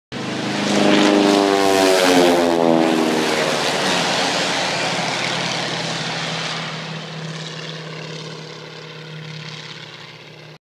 Starduster 2 flyover
Clare J. Flyover at Hawley Heights. Nothing like the sound of a radial.